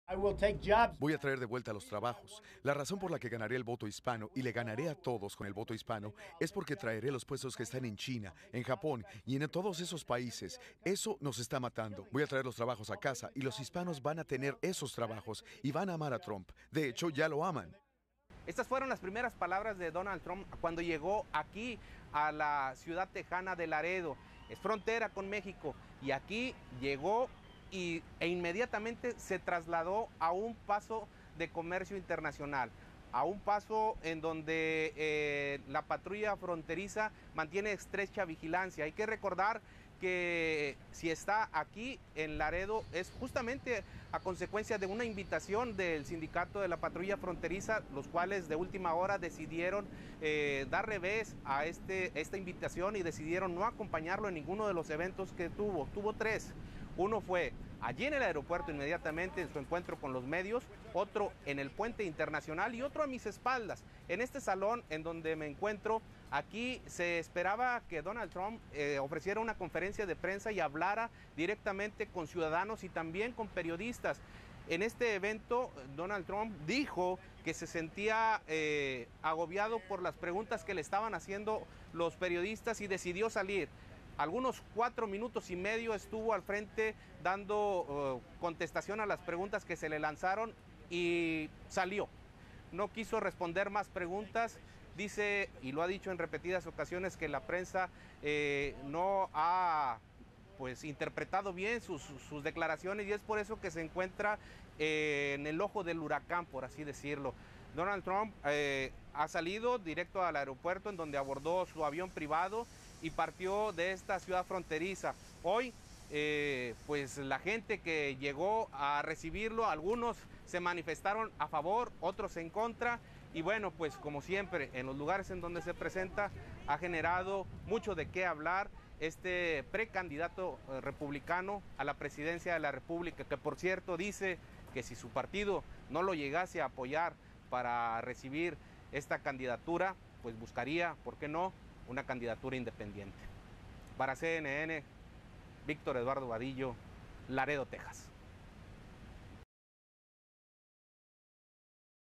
Donald Trump dice que traerá de regreso a EU los empleos que están en China y Japón
El empresario, que ha causado polémica con sus declaraciones sobre los migrantes, tuvo tres eventos en la frontera de México con Estados Unidos.